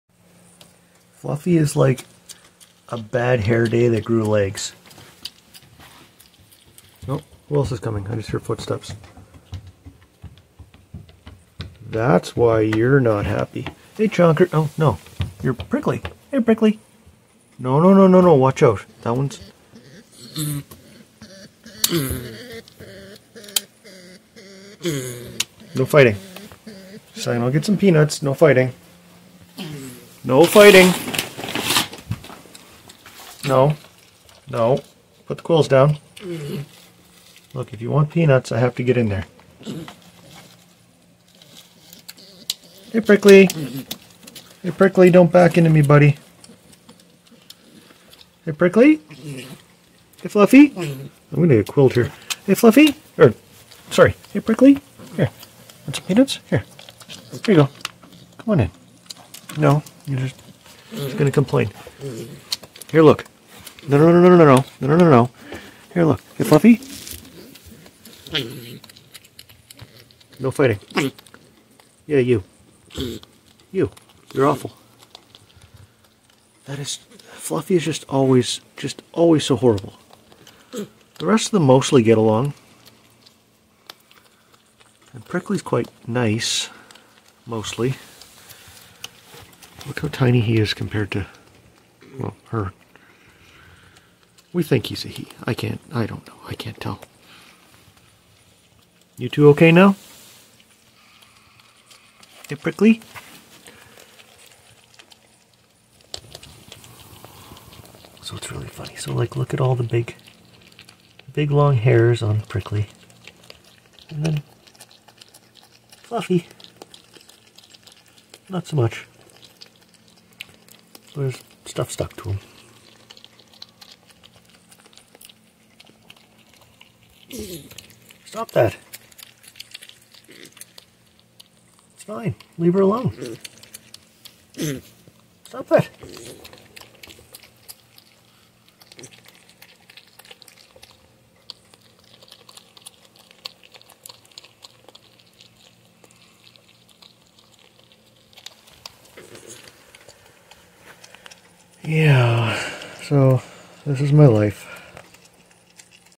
Fluffy being mean to other porcupines, as usual
Fluffy being nasty to Prickly, who is a very nice porcupine.  I don't know why Fluffy is so mean.